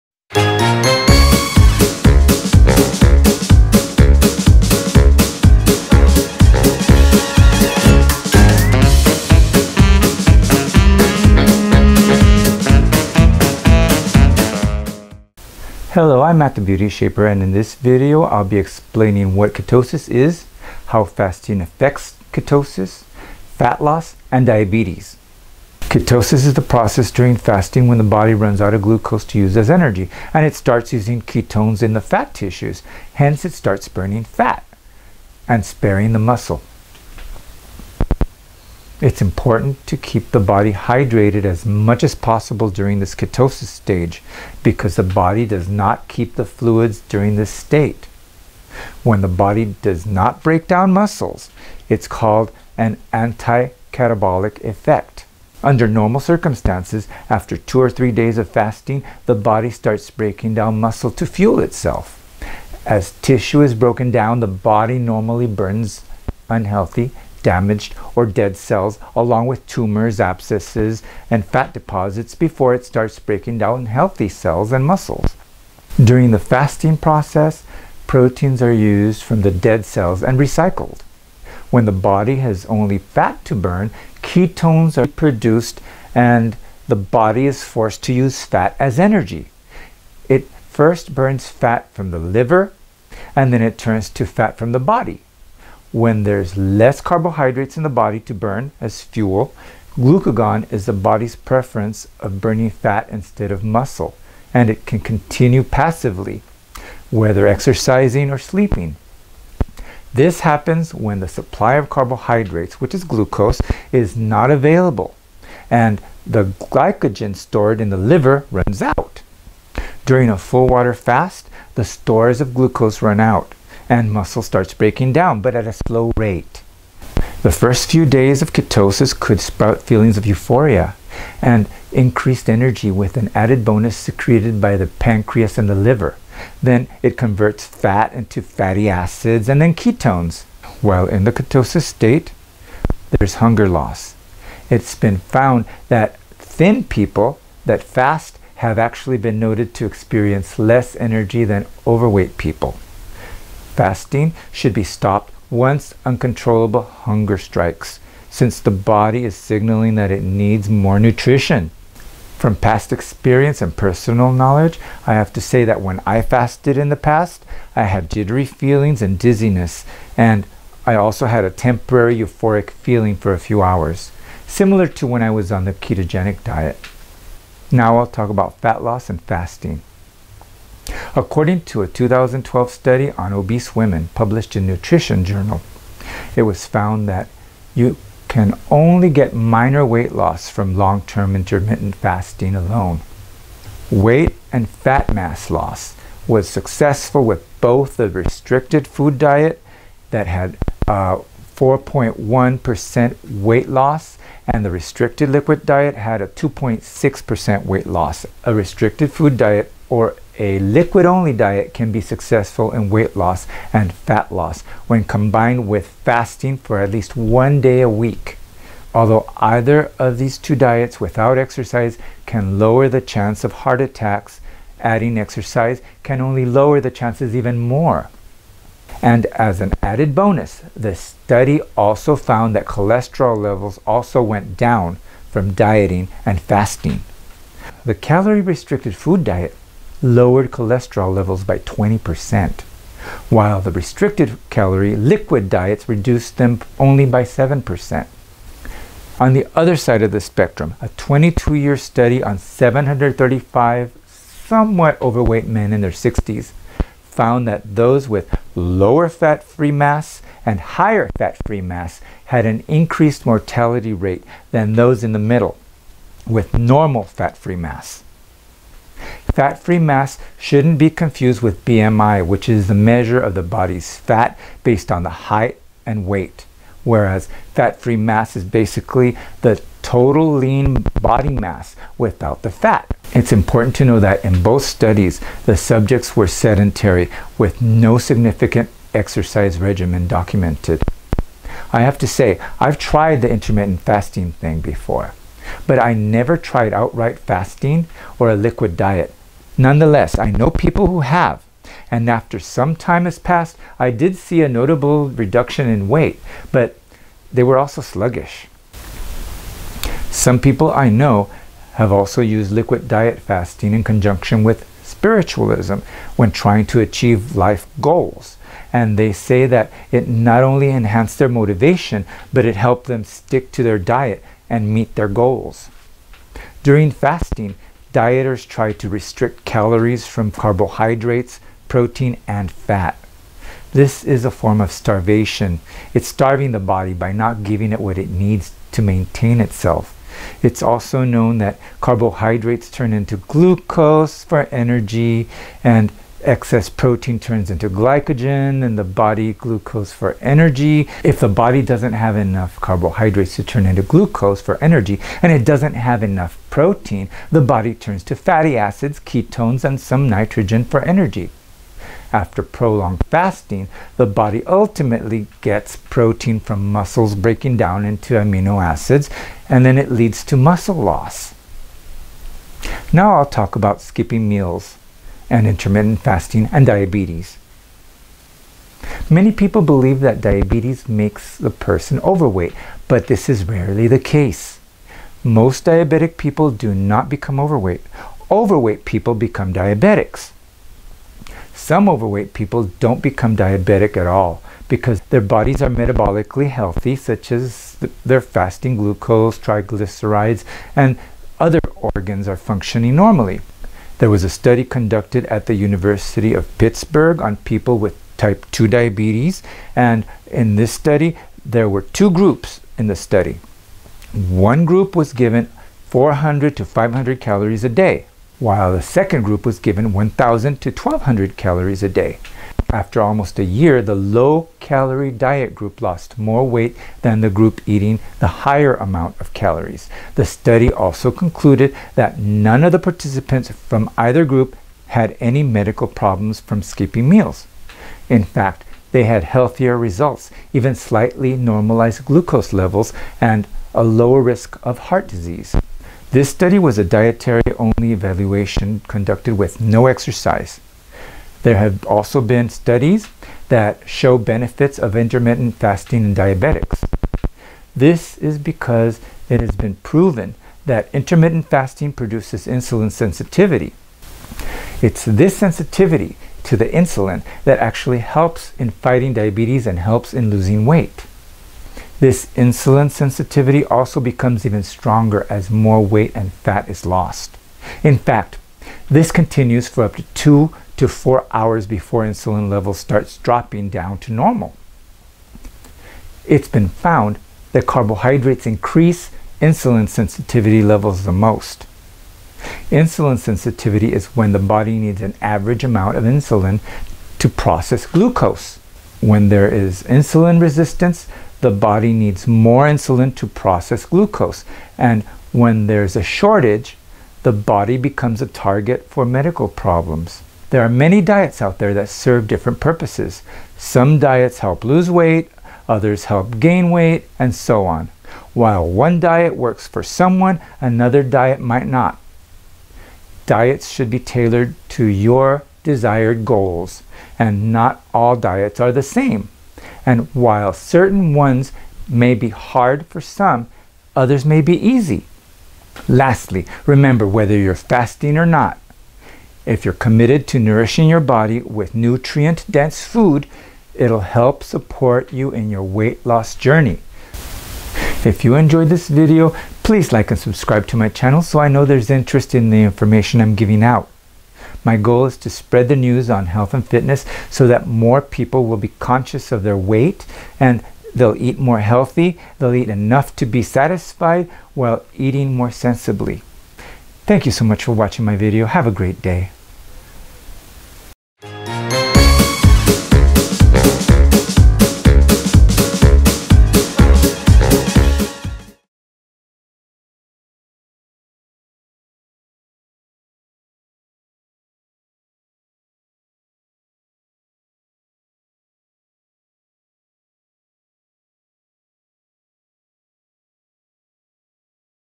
What is ketosis, Fasting and Diabetes: What Are They and How Do They Affect Your Body?
This video will unlock the secret of what ketosis is, how fasting affects ketosis, and diabetes. I will also touch upon the benefits of intermittent fasting.

Feel free to suggest a topic or idea that interests you in the comments, and I’ll be sure to incorporate it into one of my future videos.

And if you care to leave a comment of what you thought about it, I’d appreciate that too.